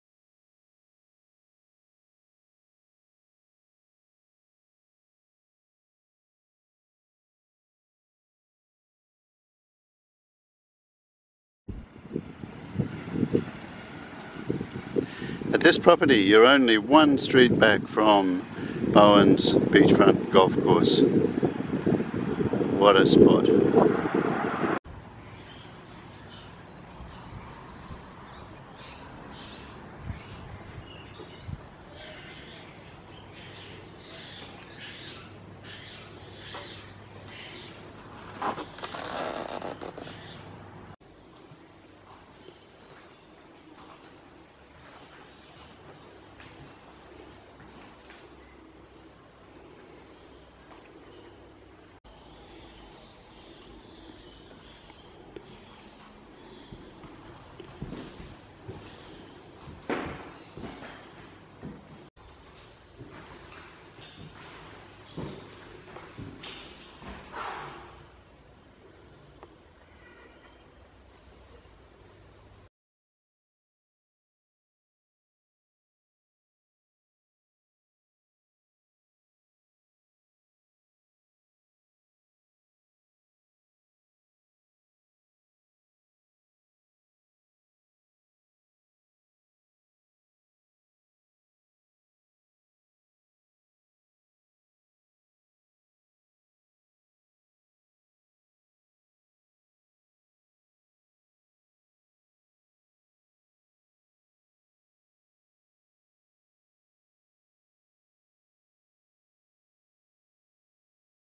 40 Field Street, Queens Beach, Bowen
Big Queens Beach home with extensive shed complex in a magical golf and beach backdrop.